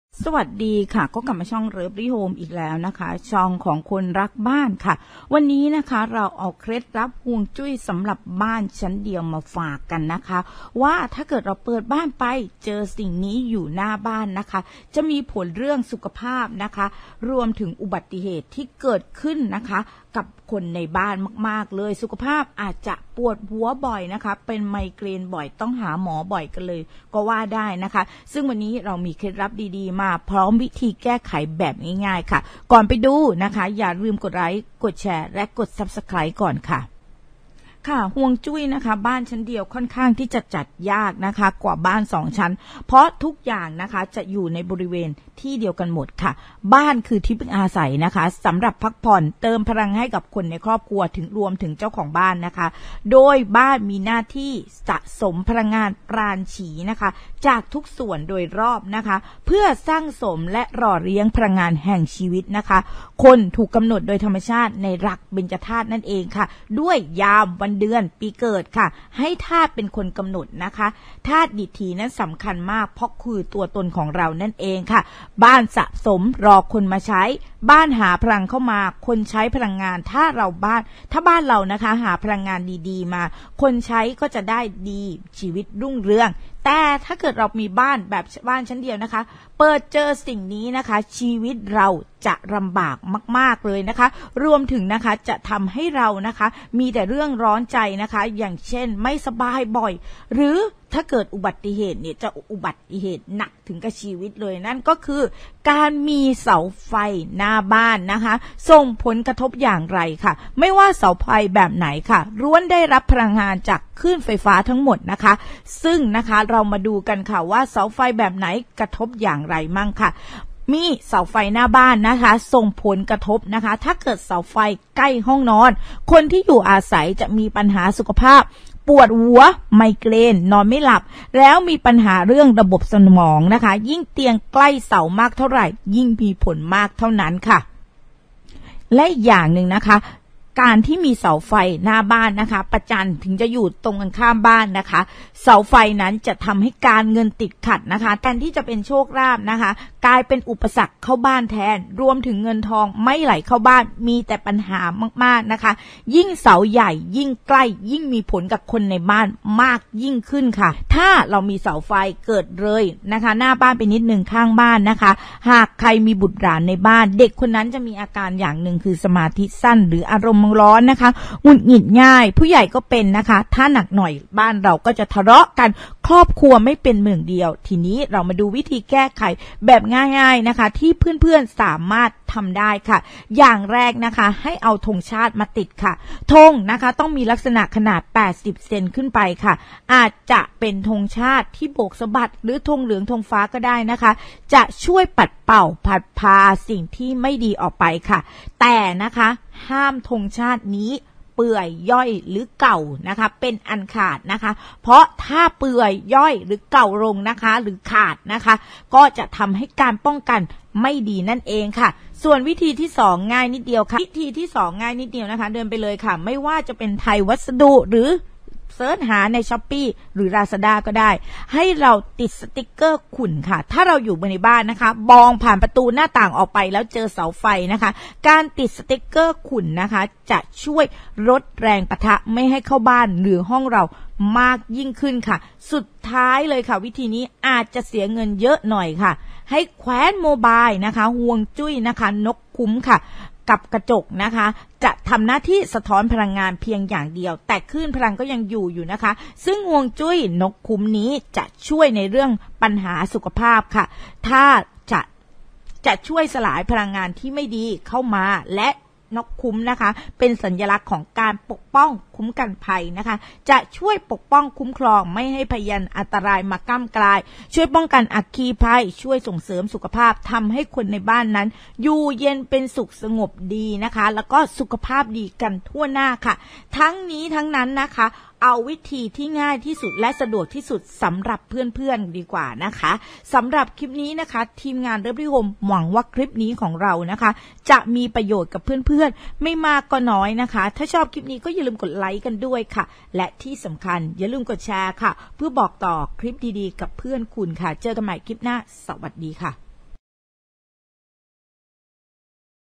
ฮวงจุ้ยบ้านชั้นนเดียว!!!เปิดมาอย่าเจอแบบนี้ สุขภาพย่ำแย่ เจออุบัติเหตุหนัก โชคลาภขาดหาย
สวัสดีค่าวันนี้เอาฮวงจุ้ยบ้านชั้นเดียวเปิดประตูมาเจอเสาไฟฟ้า ชีวิตมีปัญหา ทำให้ชีวิตไม่ราบรื่น สุขภาพย่ำแย่ โชคลาภไม่เข้าบ้าน มีปัญหาเยอะจนแยกทางกันไปดูวิธีแก้ไขกันค่า